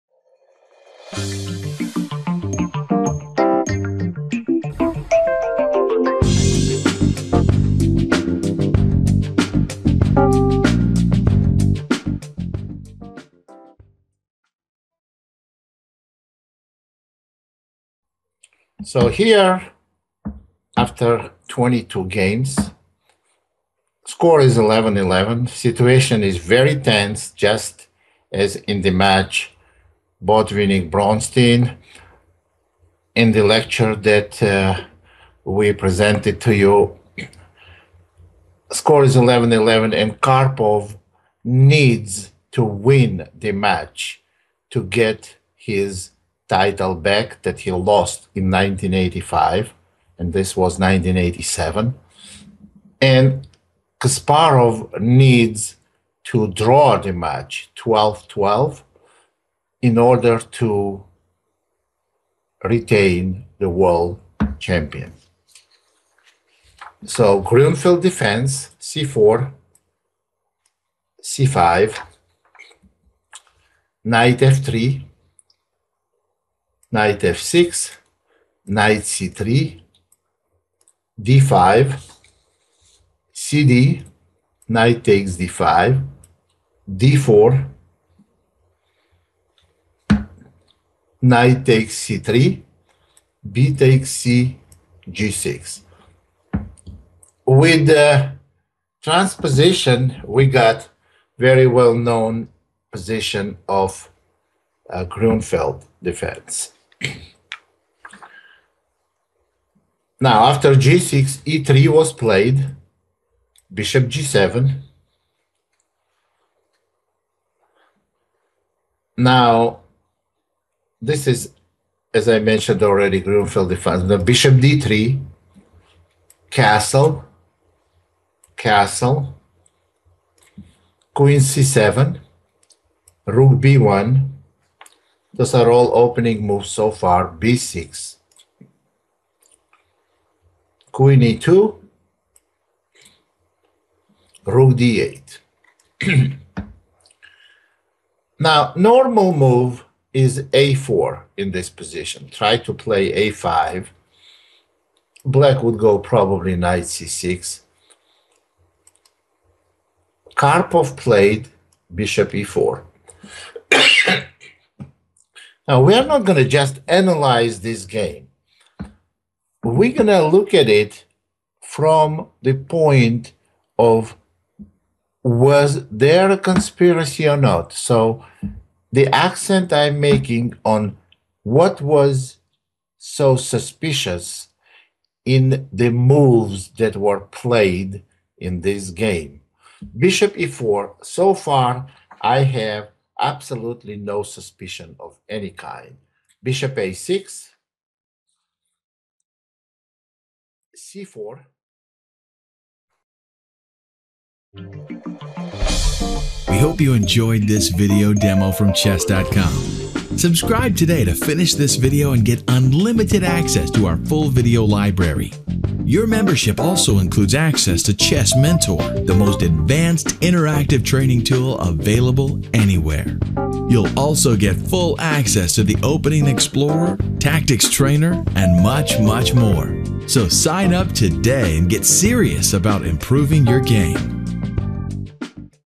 Chess.com - Most Mysterious World Championship Match 2
GM Dzindzichashvili takes a microscope to the final two games of the Kasparov-Karpov clash of 1987. Not only analyzing the moves to understand chess better, but also to consider how we can understand the mistakes made.